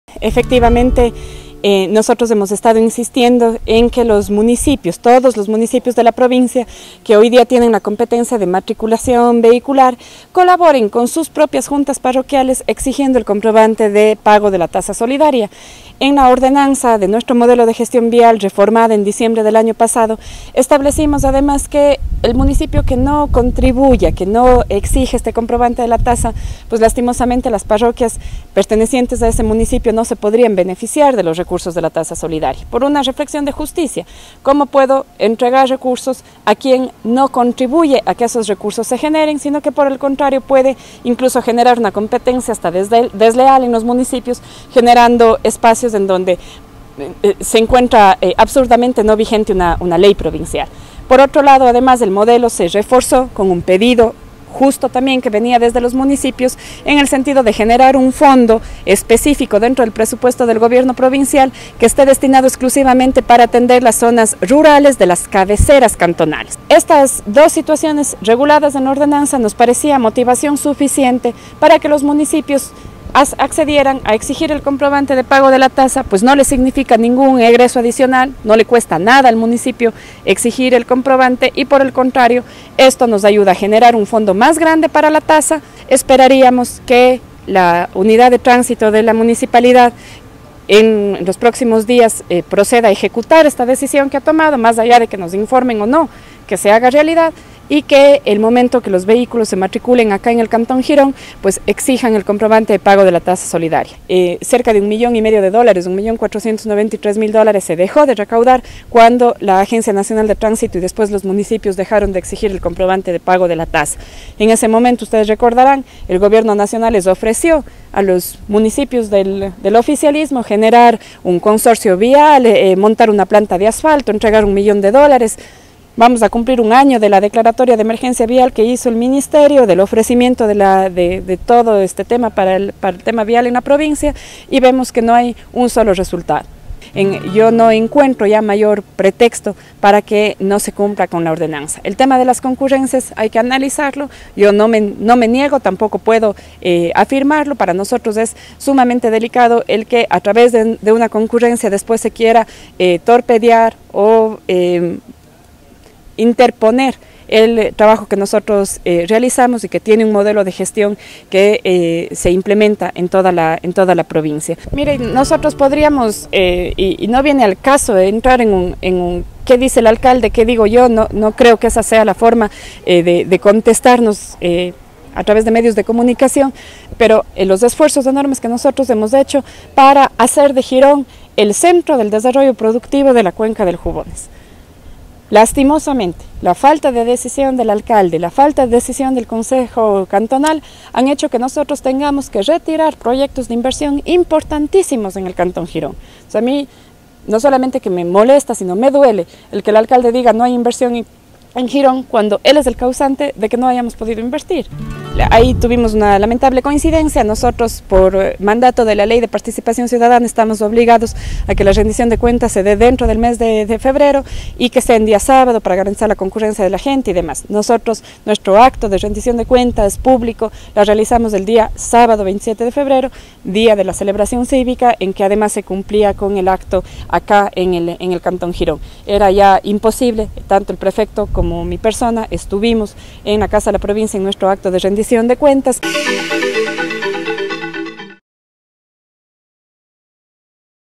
Vice prefecta del Azuay se refirió a temas relacionados a Girón
Aprovechamos la presencia de la vice prefecta del Azuay en las instalaciones de la fábrica de lácteos LACTJUBONES para consultarle sobre aspectos concernientes al cantón Girón: tasa solidaria que fu autorizado su cobro por el concejo de Girón, firma de concurrencias pedida por el GAD Municipal de Girón, falta de inversión de la prefectura en Girón y la no asistencia a las conmemoraciones del 27 de febrero que fue reclamada por el gobernador del Azuay en la sesión solemne del 27 de febrero.